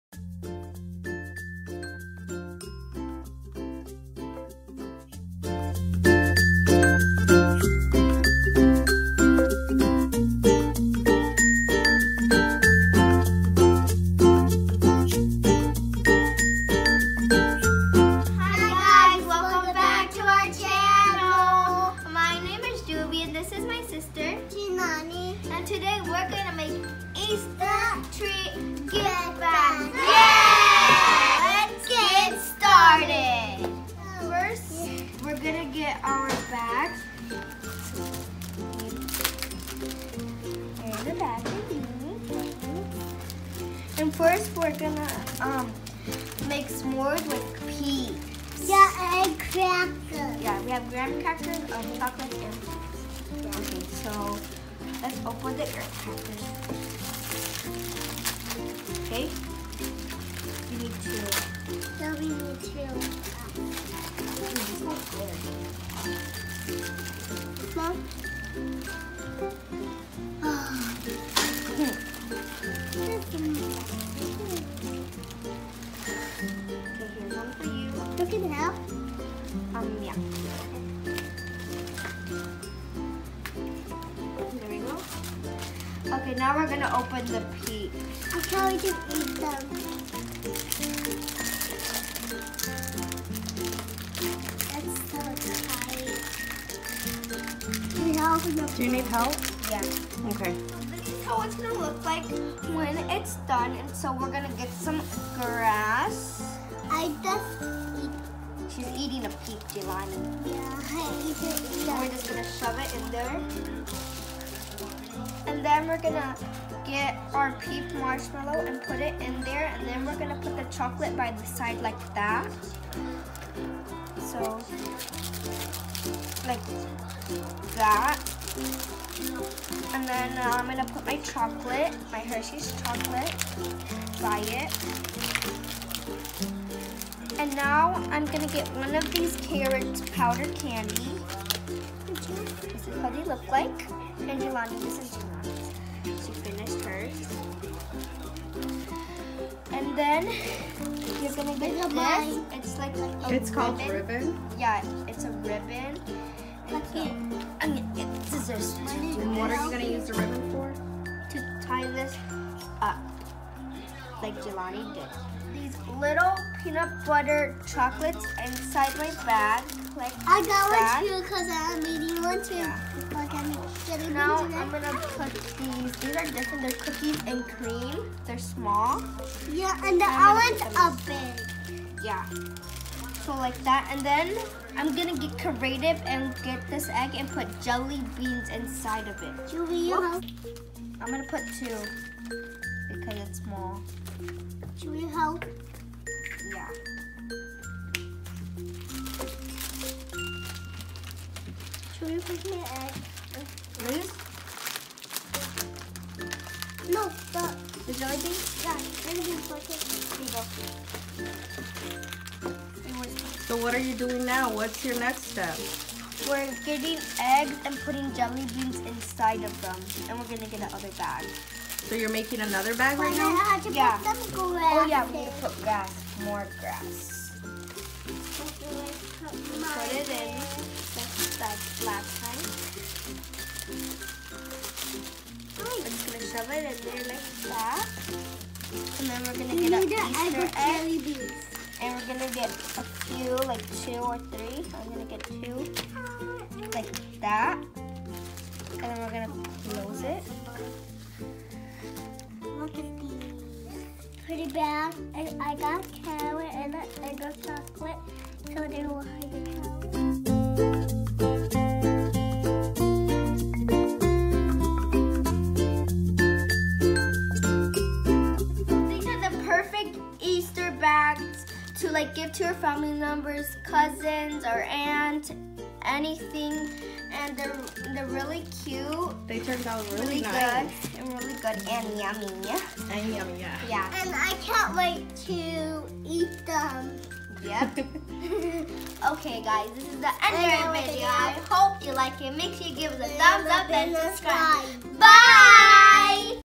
EASTER 🐣 PEEPS🐣TREAT BAGS! HOW TO MAKE (S'MORES) USING $1 ITEMS! 2018
EASTER2018 WATCH ! JJ SISTERS MAKE EASTER TREAT BAGS USING $1 ITEMS !!!